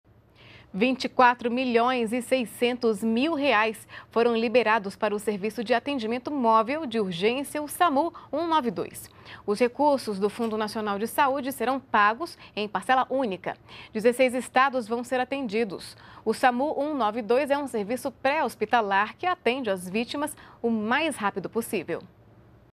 Serviço de Atendimento Móvel de Urgência recebe R$ 24,6 milhões para custeio de atividades
NBR NOTÍCIAS - 01.06.15: Nesta segunda-feira (1º),  o Ministério da Saúde liberou R$ 24,6 milhões para custeio do Serviço de Atendimento Móvel de Urgência (Samu 192). Os recursos, do Fundo Nacional de Saúde, serão pagos em parcela única e irão beneficiar 16 estados.